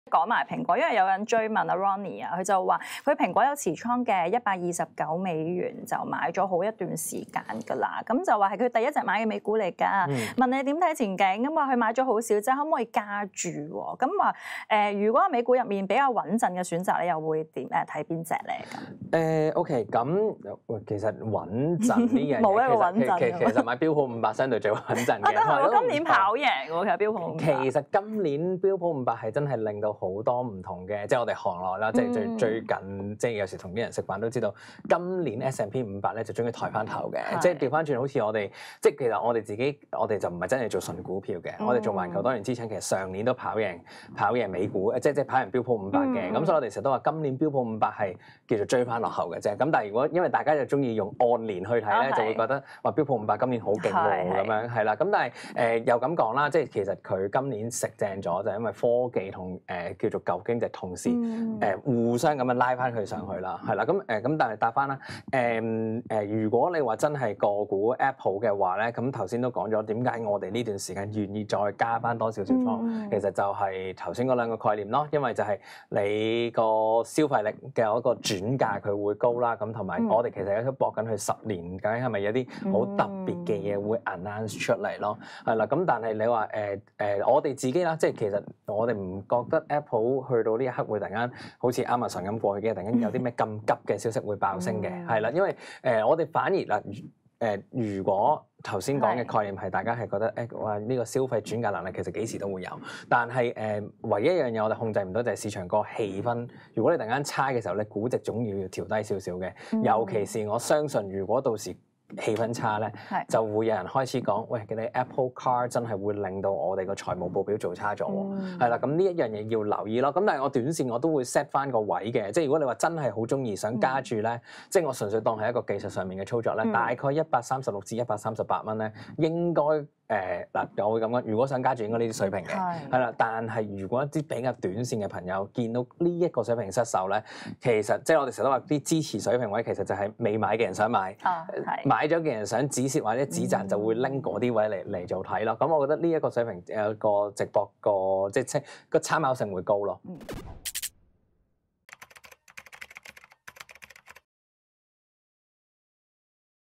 【ET開市直擊】（精華）蘋果股票唔怕買 買賣操作怎部署？
蘋果公司（Apple Inc）不怕買？持有蘋果股票的投資者欲加注，又或是沒有貨的投資者想追入，哪個價位買入蘋果股票會較理想？LW A.M.基金經理/合夥人黃耀宗（Andy）為你分析.